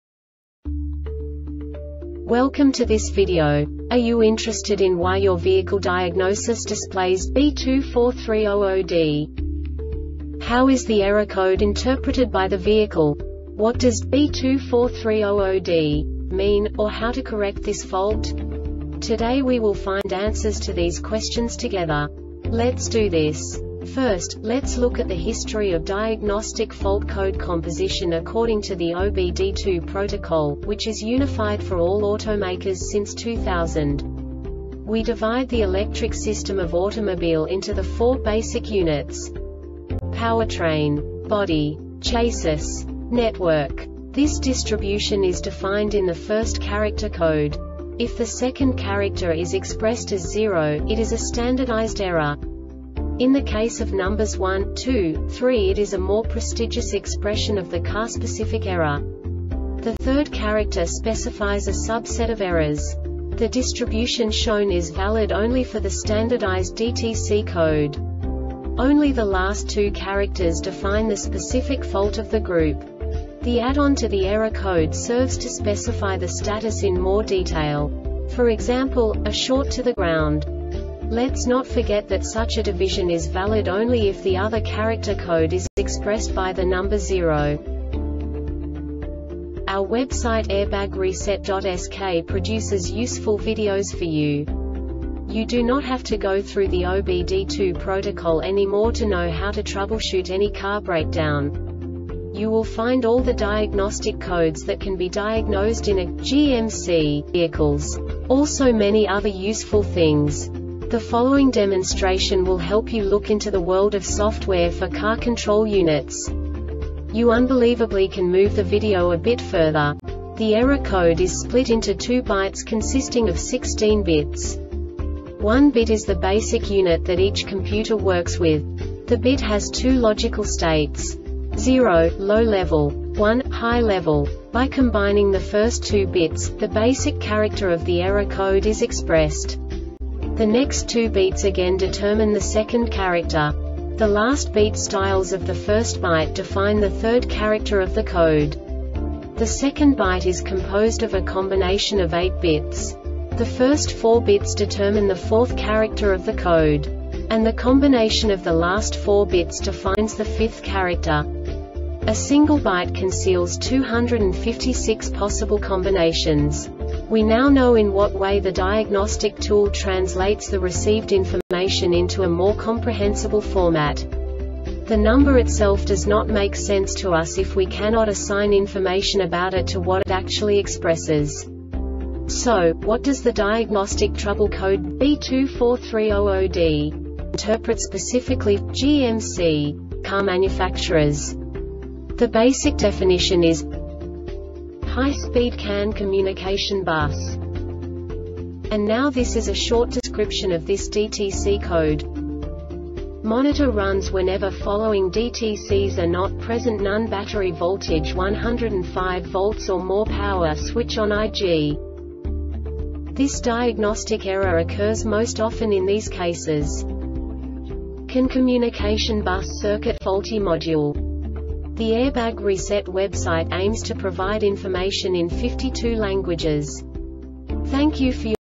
DTC GMC B2430-0D Short Explanation
Contents:
0:21 Basic DTC analysis according to OBD2 protocol standard.
1:48 Insight into programming
2:58 A diagnostically understandable description of the Diagnostic Trouble Code
3:05 Basic error definition

Make:
Code:
B2430 0D
Definition:
High Speed CAN Communication Bus
Description: